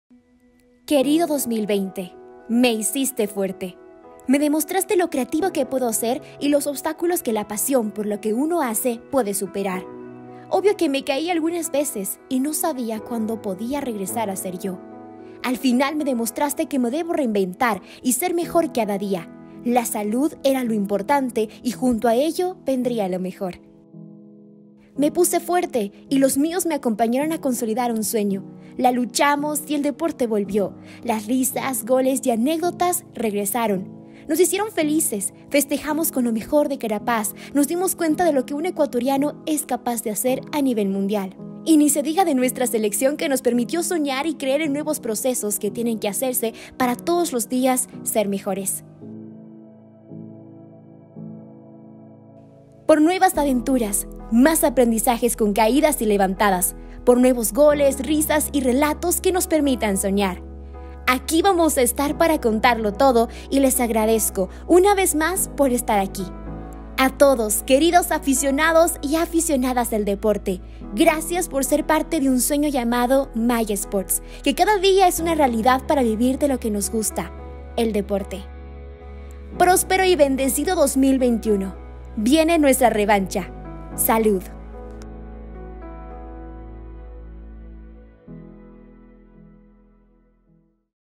ADIÓS 2020...HOLA 2021 | ¡GRACIAS POR ESTAR AQUÍ! | MAI SPORTS
Aficionados y aficionadas del deporte hoy en MaiSports les quiero agradecer por todo el apoyo que me han brindado para iniciar mi canal de youtube. Les deseo un feliz AñoNuevo2021.

Por nuevas aventuras, más aprendizajes con caídas y levantadas. Por nuevos goles, risas y relatos que nos permitan soñar. Aquí vamos a estar para contarlo todo y les agradezco una vez más por estar aquí. A todos queridos aficionados y aficionadas del deportes, gracias por ser parte de un sueño llamado Mai Sports que cada día es una realidad para vivir de lo que nos gusta el deporte

Próspero y bendecido 2021, viene nuestra revancha…¡Salud!

Gracias por ser parte de esta familia MaiSports. Nos vemos esta semana con videos nuevo.

¡Voy a leer sus comentarios!